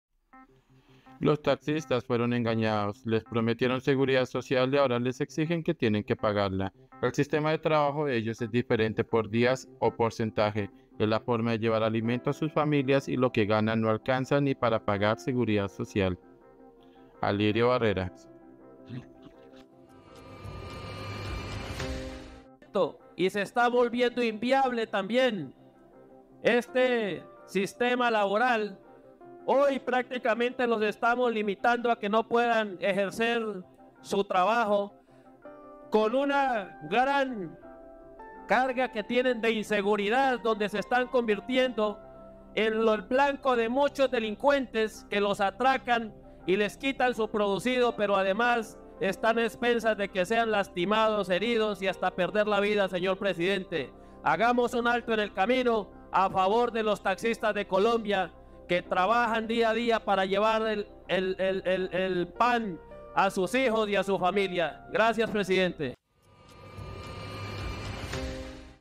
LOS TAXISTAS FUERON ENGAÑADOS 🔴 Alirio Barrera
Los TAXISTAS FUERON ENGAÑADOS
Les prometieron seguridad social y ahora les exigen que tienen que pagarla, el sistema de trabajo de ellos es diferente por días o porcentaje, es la forma de llevar alimento a sus familias y lo que ganan no alcanza ni para pagar seguridad social.
Alirio Barrera